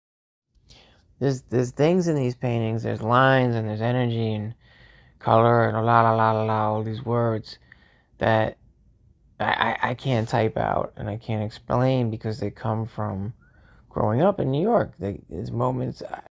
Pen and ink drawing the trees at Washington Square Park NYC
NYC art Michael Alan Alien